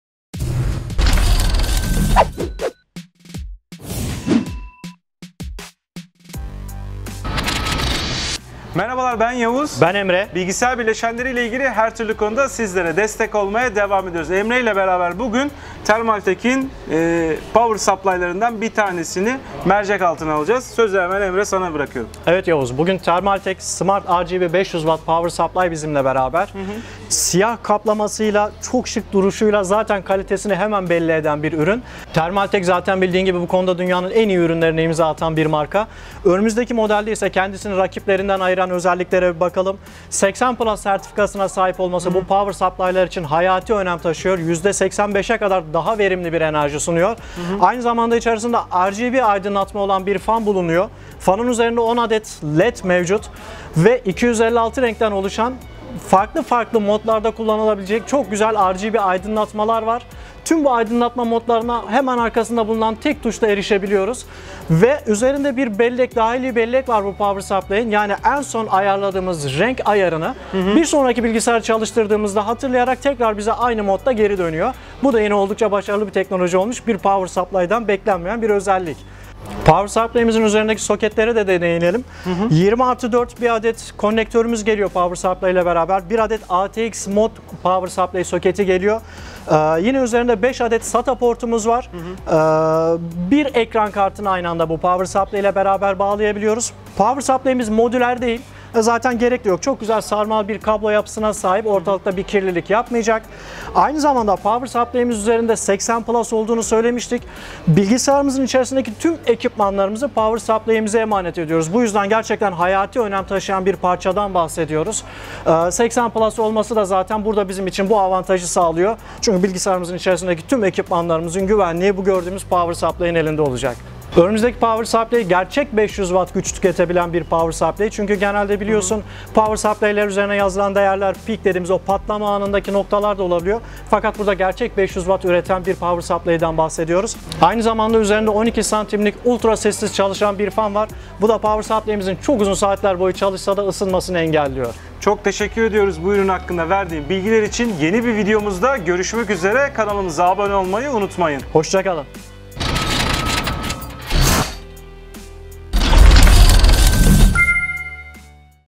Thermaltake Smart RGB 500 W Güç Kaynağı İncelemesi |MediaMarkt
Son teknoloji ürünlerle kazananlar MediaMarkt'ta! MediaTrend ekibimiz Thermaltake Smart RGB 500 W güç kaynağını sizler için inceledi! OEM ürünlerimizi incelemek için tıkla.

Avrupa'nın 1 Numaralı Elektronik Perakendecisi Media Markt'ın Resmi YouTube Sayfası